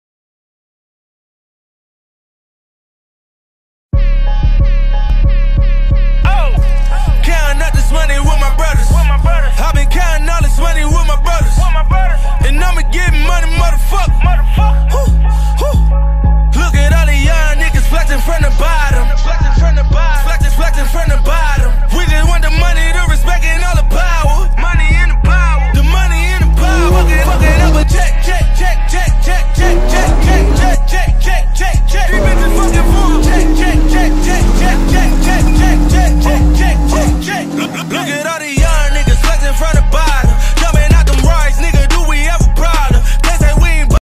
Meek Mill Brings out the bikes during CIAA 2017
Rapper Meek Mill pulls up to Charlotte, NC for CIAA 2017 on a 4 wheeler with his team following behind on their bikes.